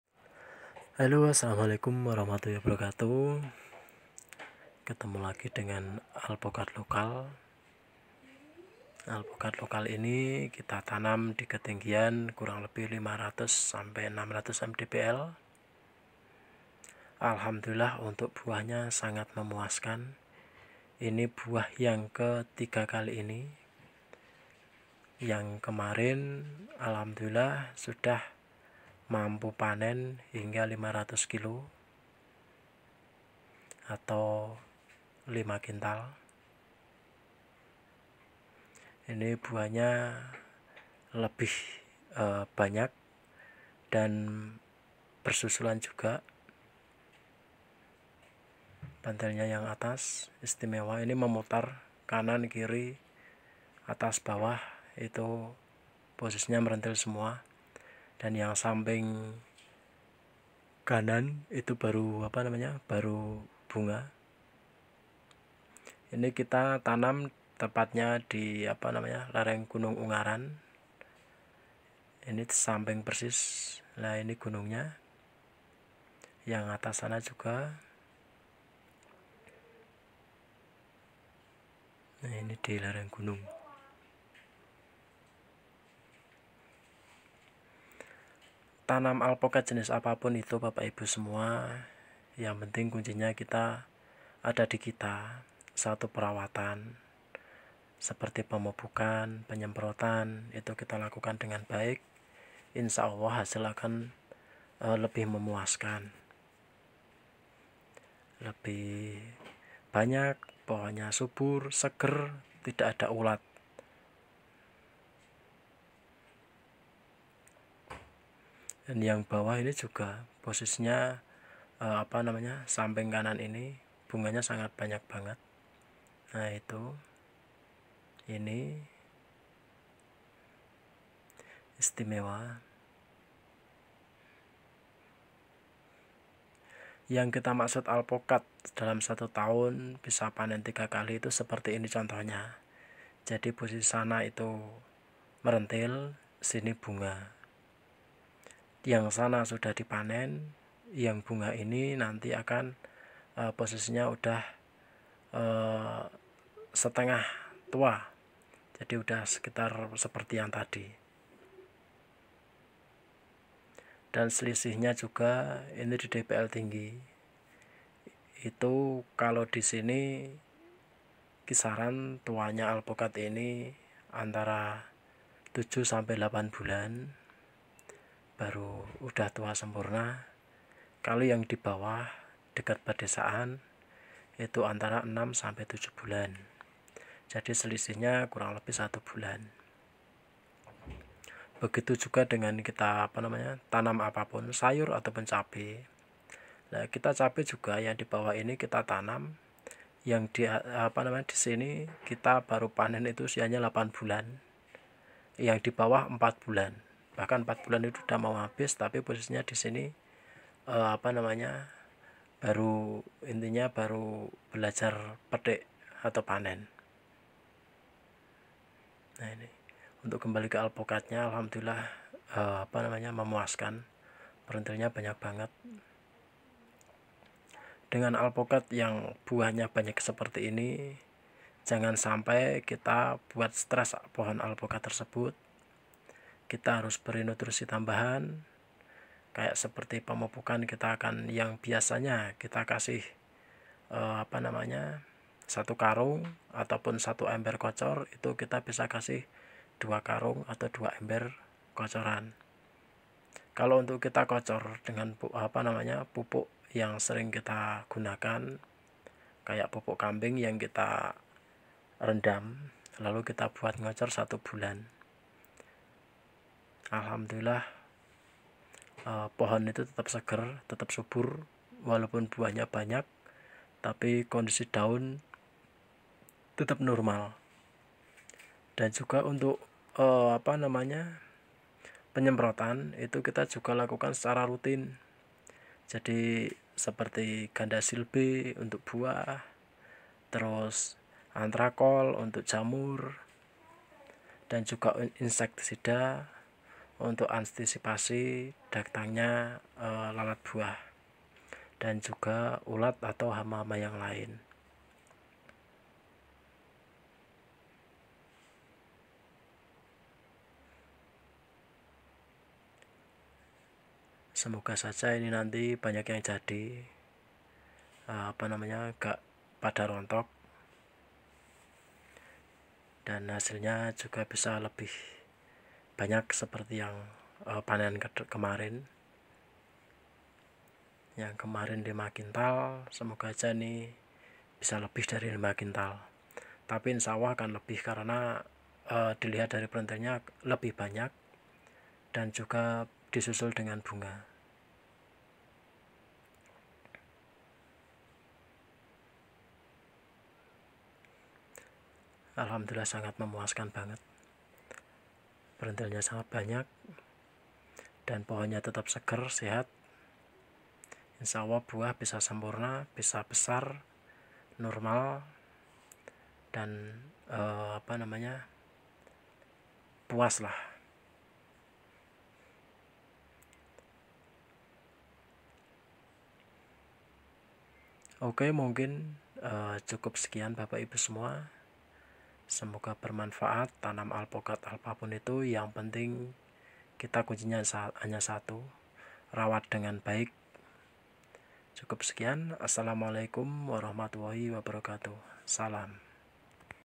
Alpukat Lokal Di MDPL 1500-1600an
Berkebun Alpukat lokal Alhamdulillah walaupun hanya sekedar lokal
Buahnya membagakan, pada waktu panen kemaren pohon usia kurang lebih 7tahun ini sudah mampu menghasilkan 500kg
Buah lumayan besar berat normal Hingga sampai 1kg
Dan alpokat ini lumayan kuning pada waktu panen di musim kemarau karakter buahnya kering pulen dan sedikit kurang berisi
Dan pada musim hujan alpokat ini berair kuning padet dan berisi
Mau tanam jenis apapun itu kuncinya hanya ada pada kita , bila kita sayang terhadap tanaman dari perawatan,pemupukan dan penyemprotan kita lakukan secara rutin Insyaallah hasil buahnya nantin akan lebih memuaskan
Salam berkebun salam alpokat Nusantara